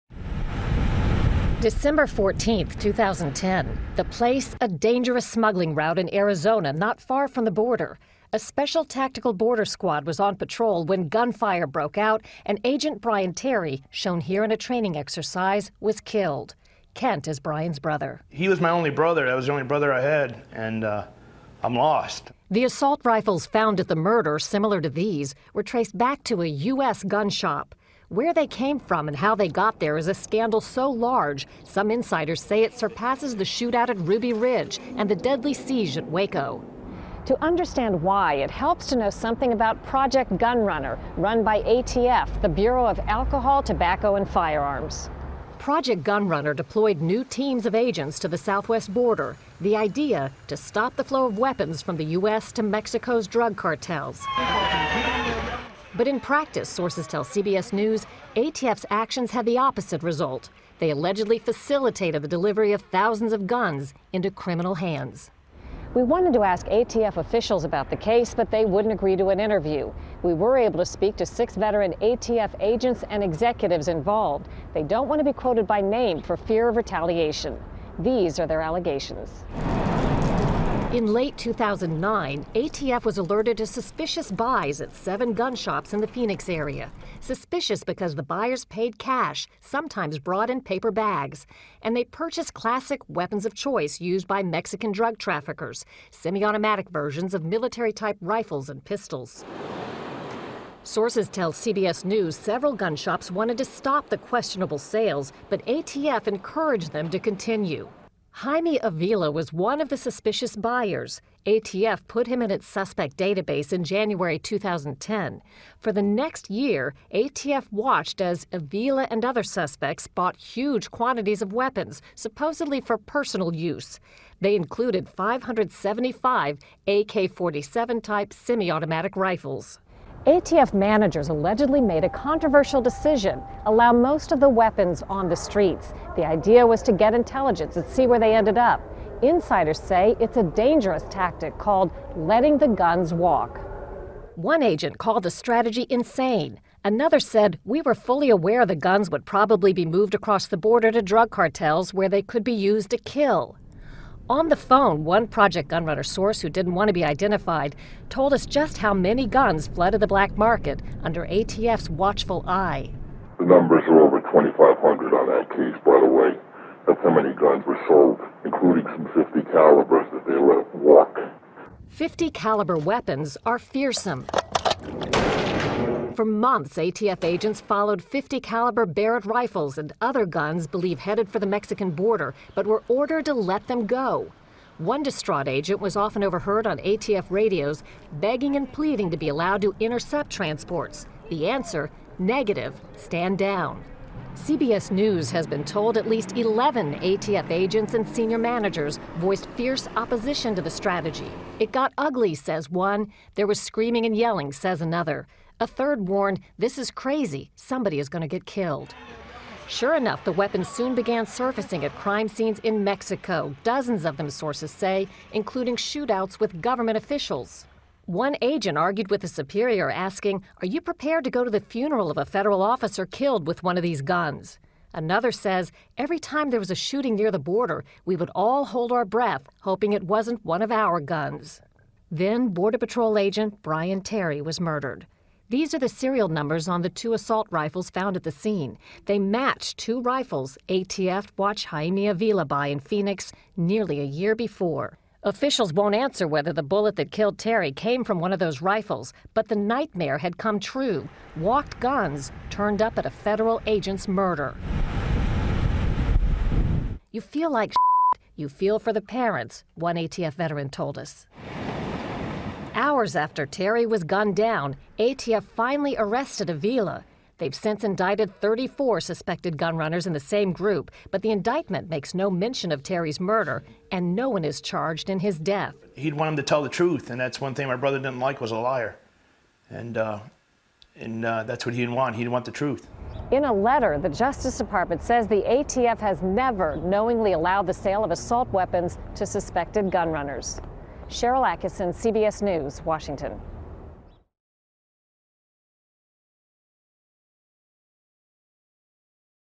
ATFE - Project Gunrunner - Scandal.flv
Ideally, Project Gunrunner was an operation conducted by the U.S. Bureau of Alcohol, Tobacco, Firearms and Explosives (ATF) intended to stem the flow of firearms into Mexico, in an attempt to deprive the Mexican drug cartels of weapons.

But evidence has surface that ATF has acted criminally by actually aiding the flow of guns illegally into the hands of NARCO-Terrorists.

And in 02/2011, ATF's actions cost the life of Border Agent Brian Terry,in which a shootout with NARCO terrorists using weapons ATF allowed them to get across the border.